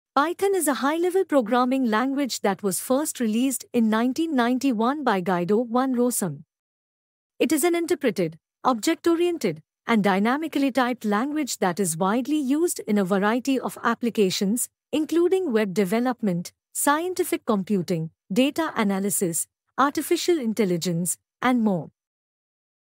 What is Python
Python is a high-level programming language that was first released in 1991 by Guido van Rossum. It is an interpreted, object-oriented, and dynamically-typed language that is widely used in a variety of applications, including web development, scientific computing, data analysis, artificial intelligence, and more.